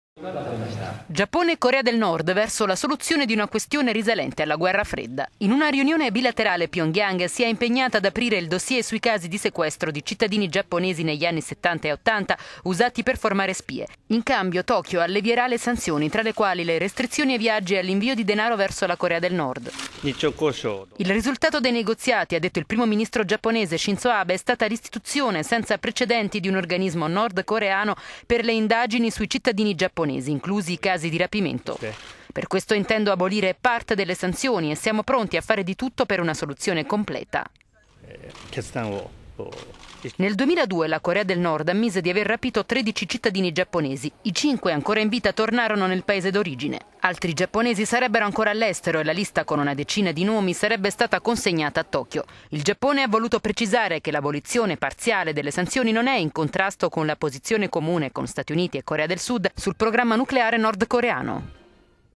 Corea del Nord riapre inchiesta su giapponesi rapiti, Tokyo allevia sanzioni
Giappone e Corea del Nord verso la soluzione di una questione risalente alla Guerra Fredda. In una riunione bilaterale, Pyongyang si è impegnata ad aprire il dossier sui casi di sequestro di cittadini giapponesi negli anni settanta e ottanta usati per formare spie, in cambio Tokyo allevierà le sanzioni, tra le quali le restrizioni ai viaggi e all'invio di denaro verso la Corea del Nord.

"Il risultato dei negoziati è stata l'istituzione senza precedenti di un organismo nordcoreano per le indagin...